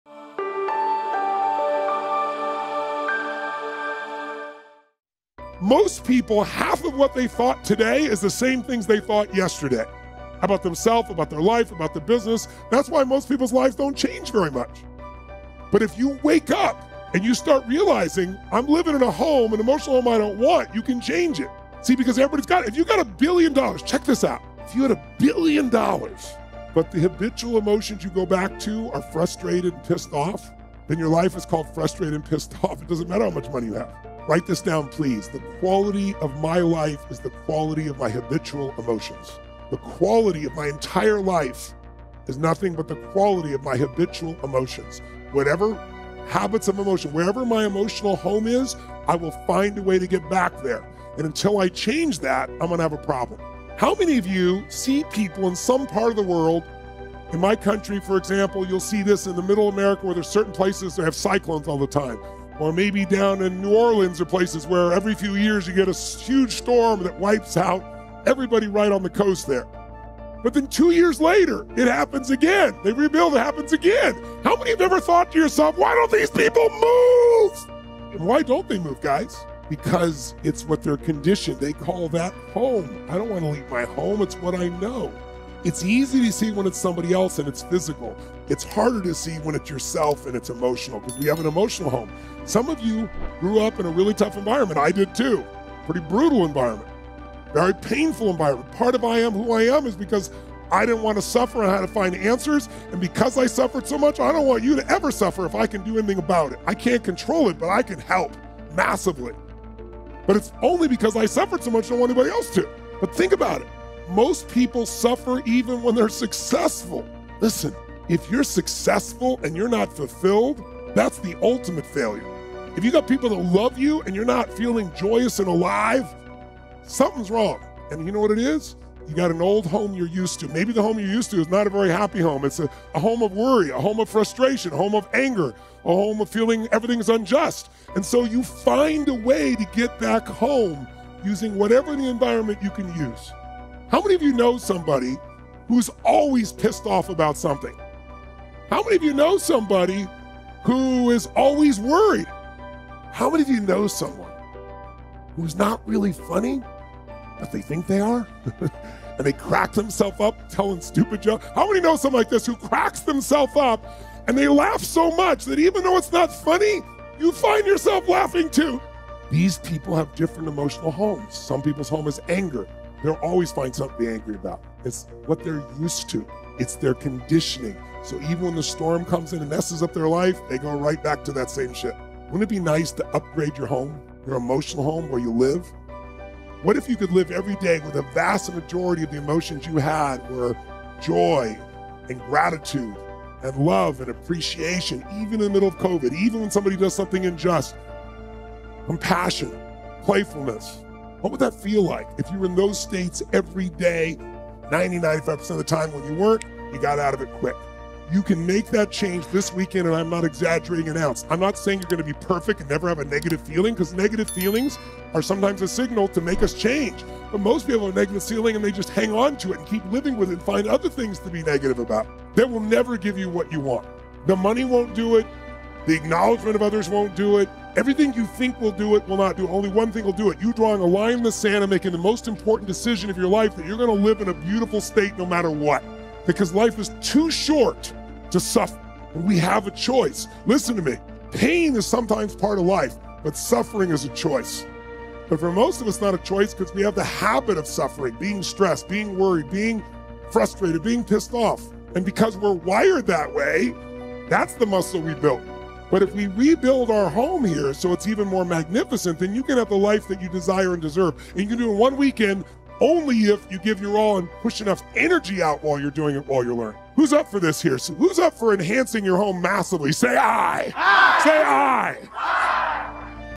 THE MOST IMPACTFUL DECISION YOU WILL EVER MAKE - TONY ROBBINS
Hey Everyone,
In this video we will share the motivational and energetic ideas of Famous motivational speaker and life coach Tony Robbins with you. We hope that this content would be very helpful in grooming and developing your life and you will think positive and will make a mindset to achieve your personal goals.